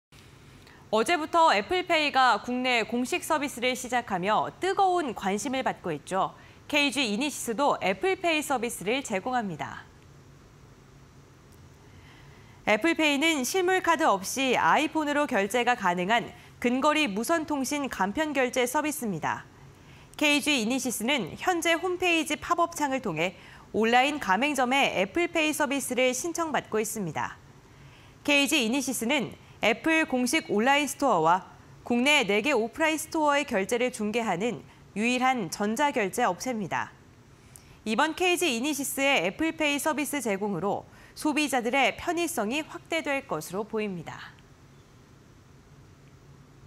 애플페이 국내 상륙... KG이니시스도 서비스 시작 (20230322)
어제부터 에플페이가 국내에 공식 서비스를 시작하며, 뜨거운 관심을 받고 있지요. KG이니시스도 애플페이 서비스를 제공합니다.

애플페이는 실물 카드 없이 아이폰으로 결제가 가능한 근거리무선통신(NFC) 간편결제 서비스입니다.

KG이니시스는 현재 홈페이지 팝업창을 통해 온라인 가맹점에 애플페이 서비스를 신청받고 있습니다.

KG이니시스는 애플 공식 온라인 스토어와 국내 4개 오프라인 스토어의 결제를 중개하는 유일한 전자결제업체입니다.

이번 KG이니시스의 애플페이 서비스 제공으로 소비자들의 편의성이 확대될 것으로 보입니다.